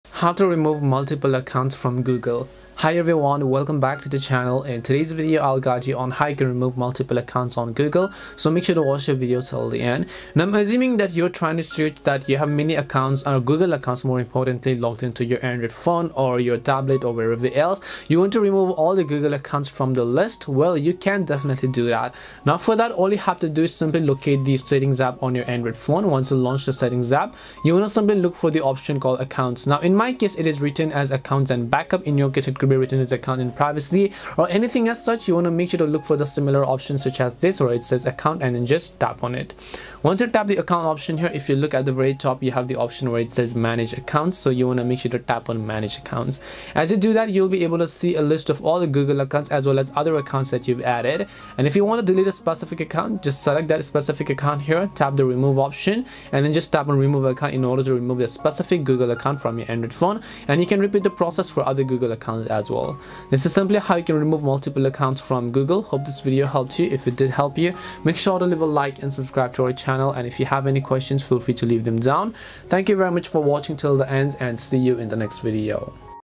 How to Remove Multiple Accounts from Google !
In this video, I will show you how to remove multiple accounts from Google.

~ Chapters:
0:00 Introduction
0:10 General Info
0:26 Remove Multiple Accounts
1:03 Conclusion